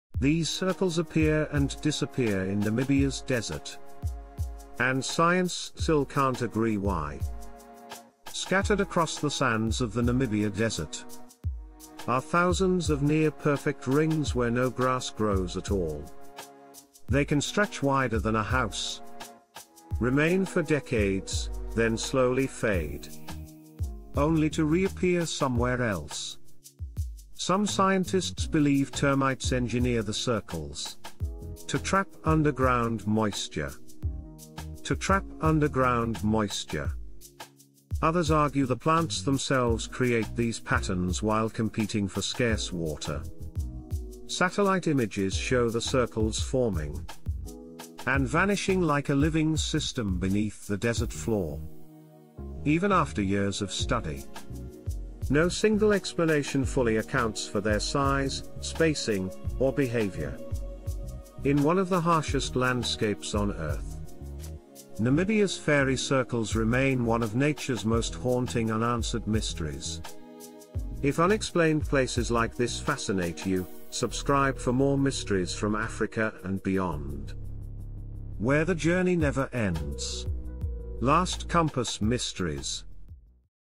Namibia’s Fairy Circles — Science Still Can’t Agree Why They Exist
Scattered across Namibia’s Namib Desert are thousands of mysterious circular patches where no grass grows. Known as Fairy Circles, these strange formations appear, disappear, and even shift over time, leaving scientists divided on what causes them.
Some researchers believe termites engineer the circles to control underground moisture, while others argue the plants themselves create self-organizing patterns in extreme drought. Even with years of study and satellite evidence, no single explanation fully accounts for their precise spacing and behavior.
Namibia’s Fairy Circles remain one of Africa’s most puzzling natural mysteries.